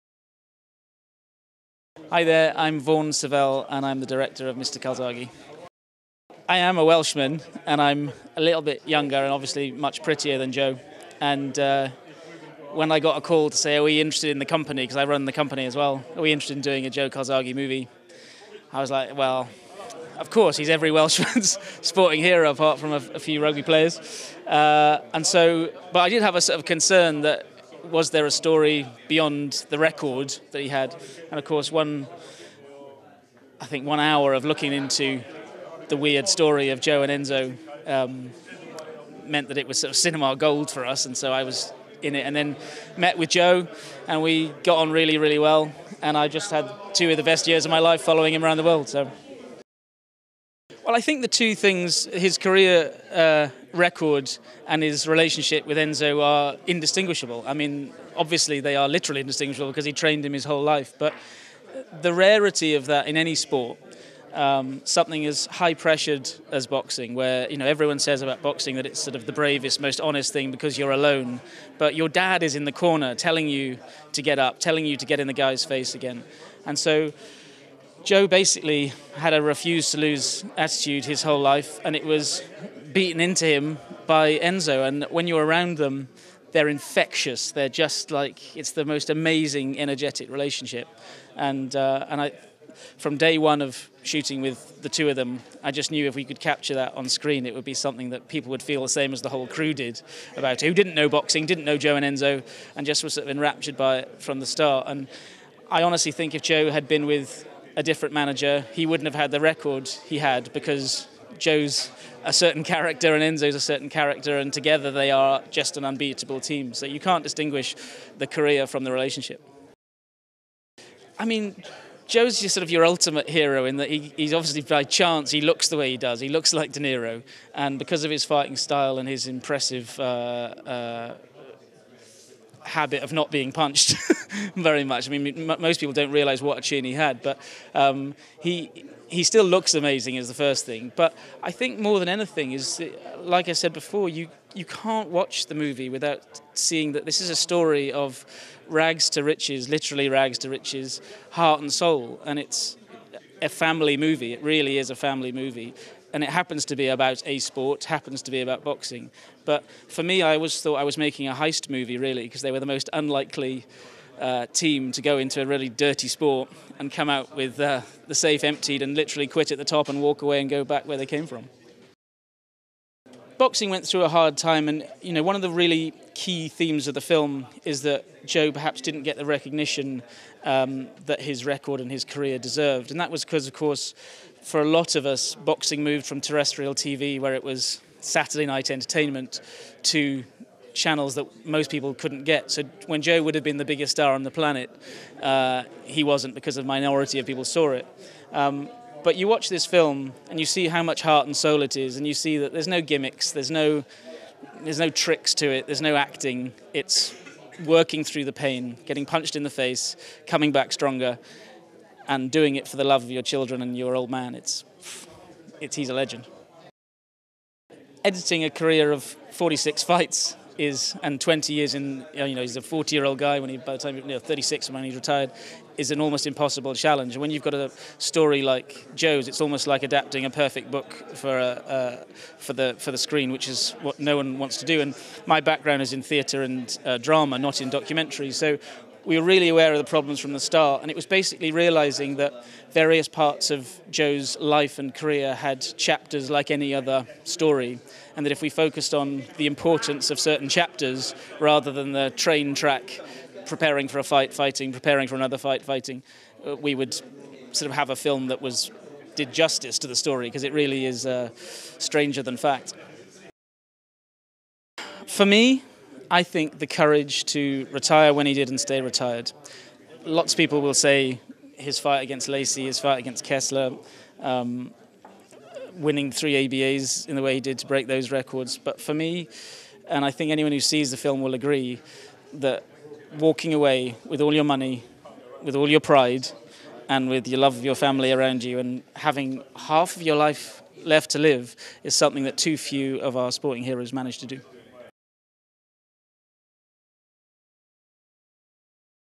Director Vaughan Sivell Interview Mr Calzaghe Premiere
Red Carpet News talks to Boxing champion Joe Calzaghe, flamboyant trainer Enzo Calzaghe and Director Vaughan Sivell at the London gala screening of documentary Mr Calzaghe.